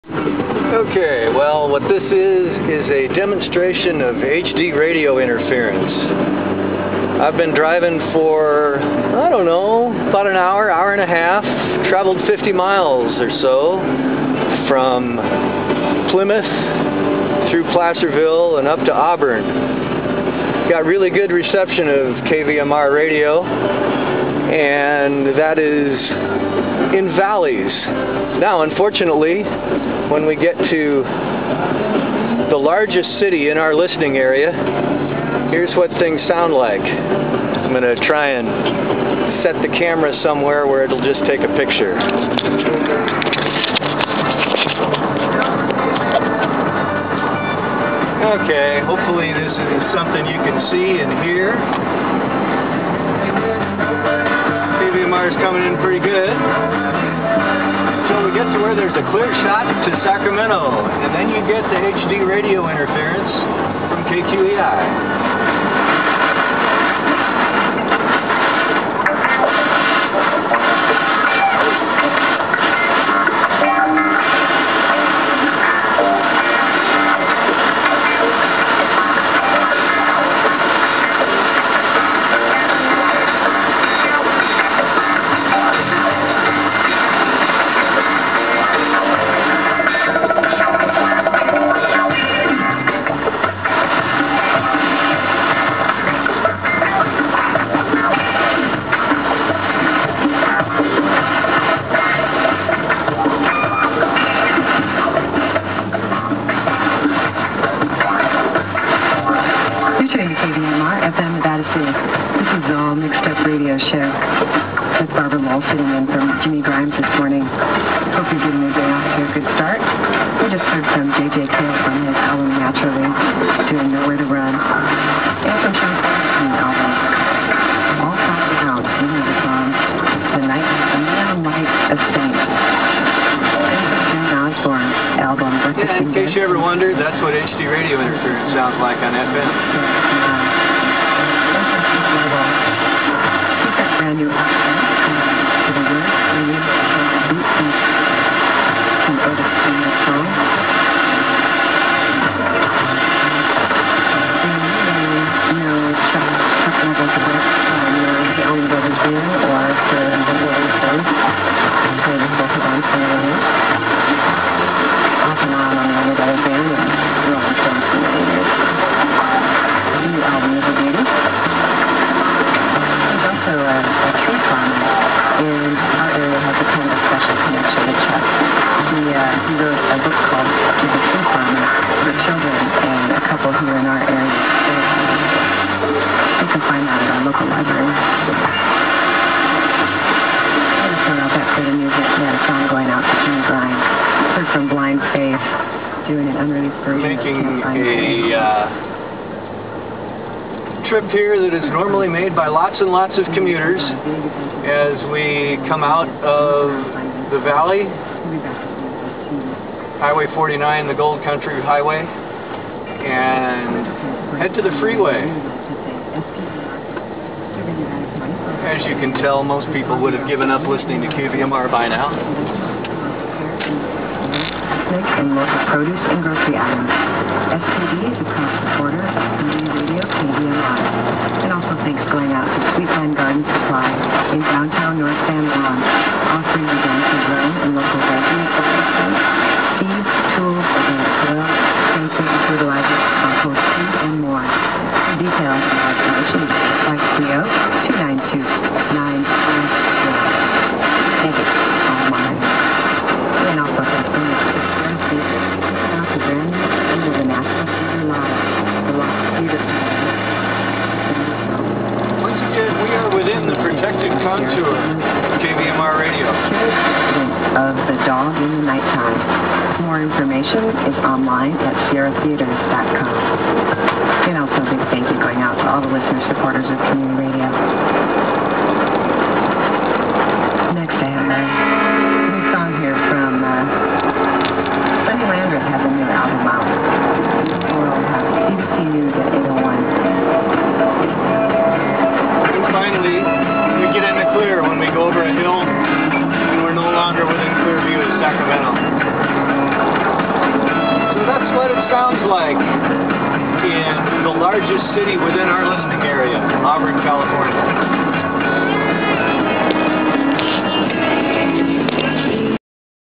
HD Radio Interference Auburn
I took this video while driving through Auburn, CA to provide some insight into what adjacent-channel HD Radio interference sounds like on FM.  It's five minutes long and the quality is not very good, but you can clearly hear what the interference sounds like. The station being received is KVMR in Nevada City on 89.5.  The interference is being caused by HD Radio on KQEI FM in Sacramento at 89.3.  The location is downtown Auburn, CA.  Like most places in the foothills, Auburn has a clear view of the Sacramento metro, and even though the interference is being generated by a fairly low-power station from 30 miles away it causes significant interference to a local FM station.  The vehicle is being driven Northbound on highway 49 into downtown and onto Interstate 80 Eastbound.  Because HD Radio interference sounds so much like simple white noise, most people simply assume that the station they are trying to listen to is just weak.  In this case, KVMR is very strong.  It's just that the interference is stronger.